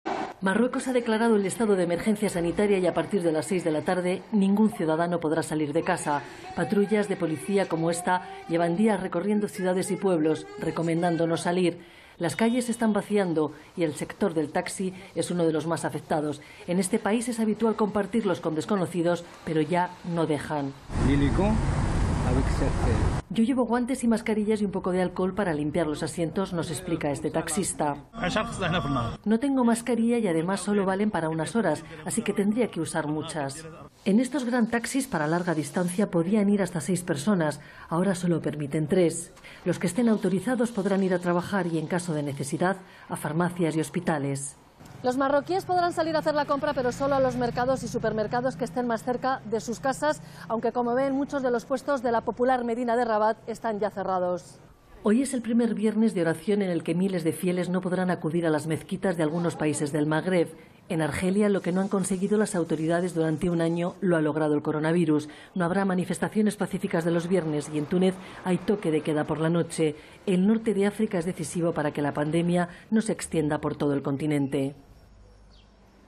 Marruecos: El país declara estado de emergencia sanitaria
Marruecos ha declarado estado de emergencia sanitaria y a partir de las seis de la tarde ningún ciudadano podrá salir de casa. Las calles se han vaciado y el sector del taxi es uno de los más afectados. En este país es habitual compartirlos con desconocidos, pero ya no dejan.